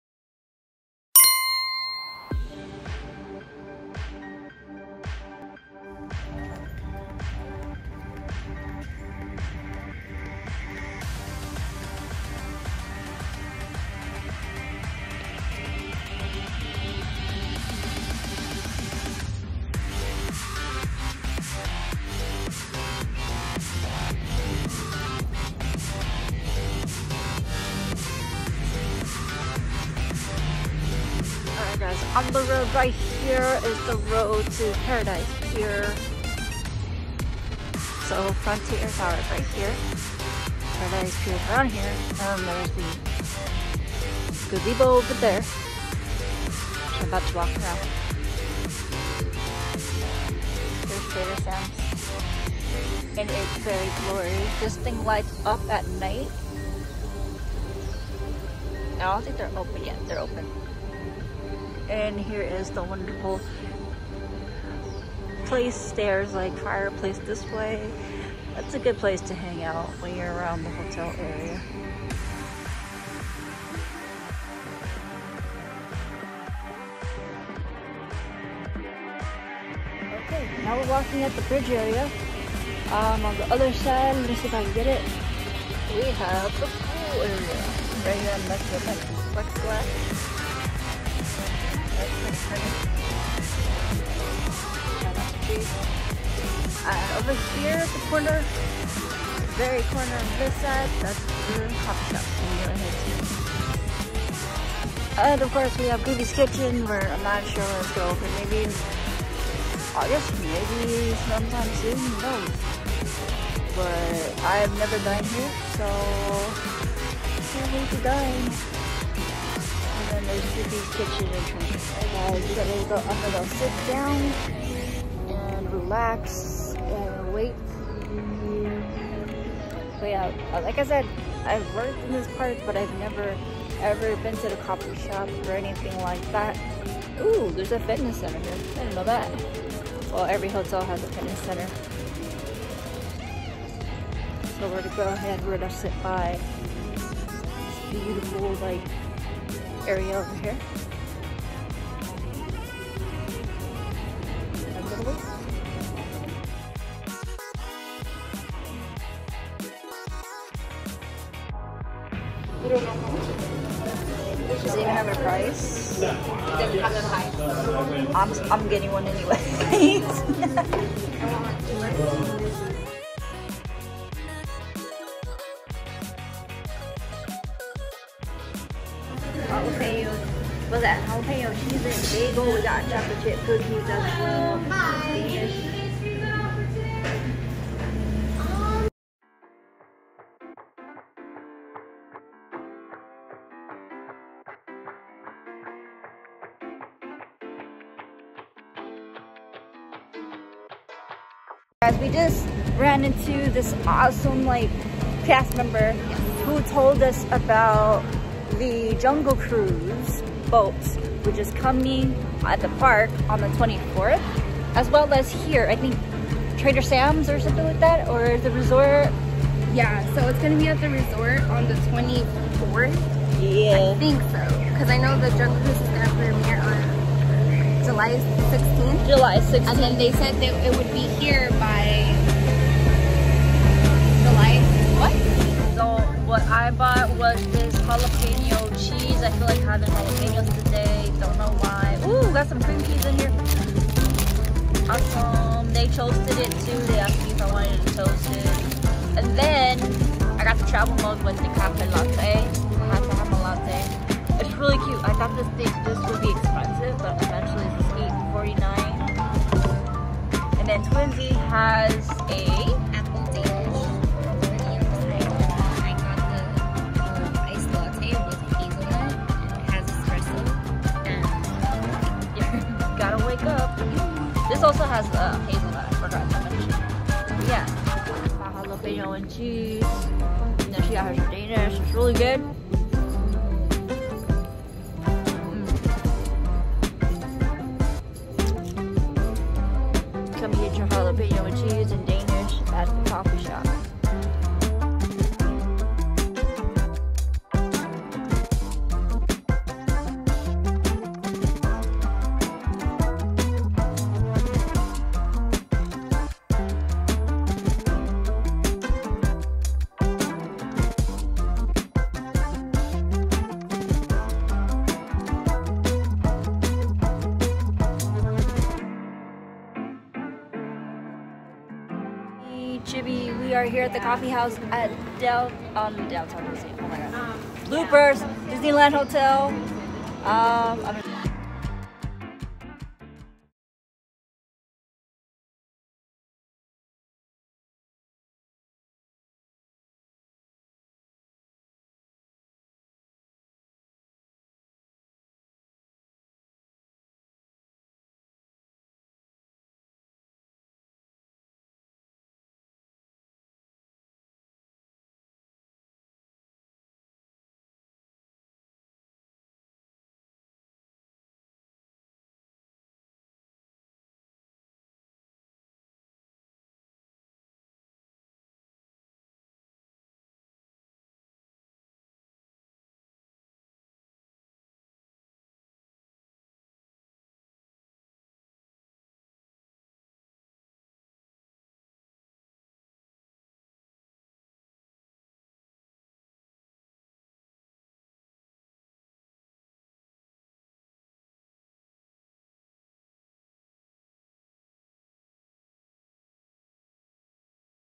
Coffee time w/Chibi and Kanani
Hey guys,
This video is so late but I want to put it up anyways! It’s coffee time with me and Kanani from AlNani Vibez. This is our experience and a little tour of the Disneyland resort while I wait. Thank your for supporting us and viewing our Channel!

See you next Vlog Venturers